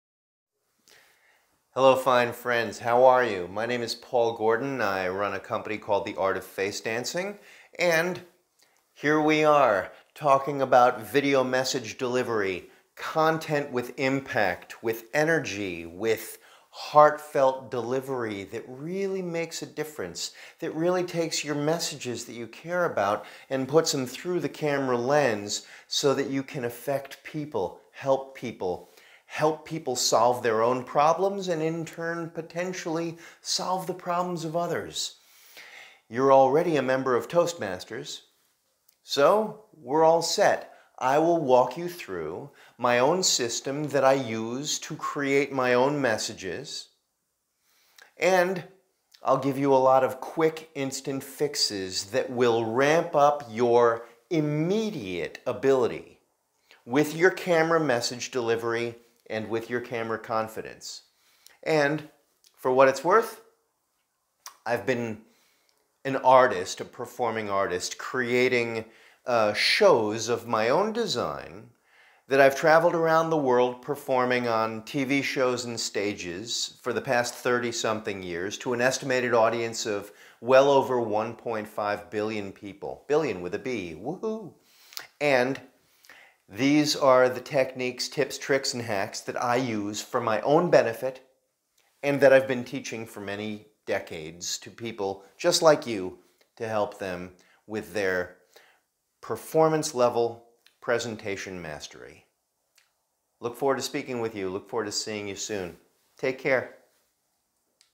Speaking to a Virtual Audience - Paul Gordon's lecture - promo video
Paul Gordon's interactive lecture for Krypton Advanced Toastmasters

In this interactive lecture, Paul Gordon, master lecturer, coach and professional international presenter/performer offers a dynamic and impactful series of instant fixes for online video mastery.
13th July, 2020 at 7:30pm